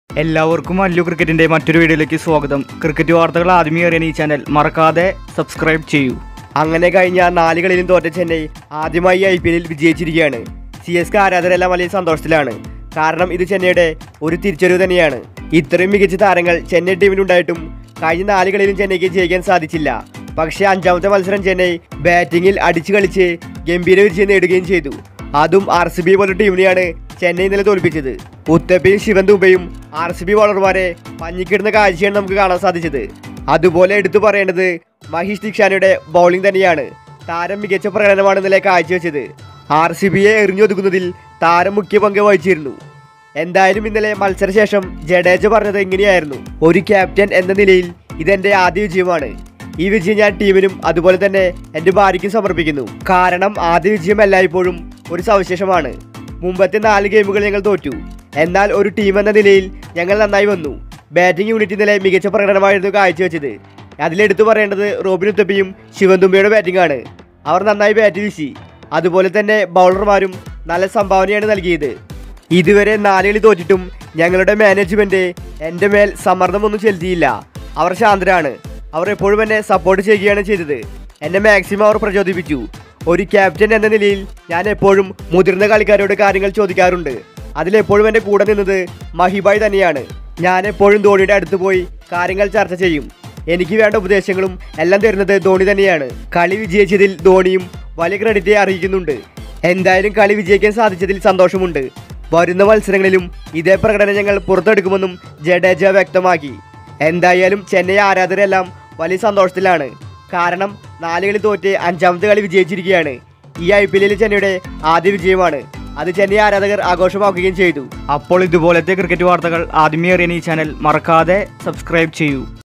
ഇതുകൊണ്ടാണല്ലേ ജയിച്ചേ ജഡേജ ആ വലിയ രഹസ്യം വെളിപ്പെടുത്തി | CSK VS RCB FULL MATCH HIGHLIGHTS | DHONI
Royal Challengers Bangalore vs Chennai Super Kings Full Match Highlights | CSK VS RCB |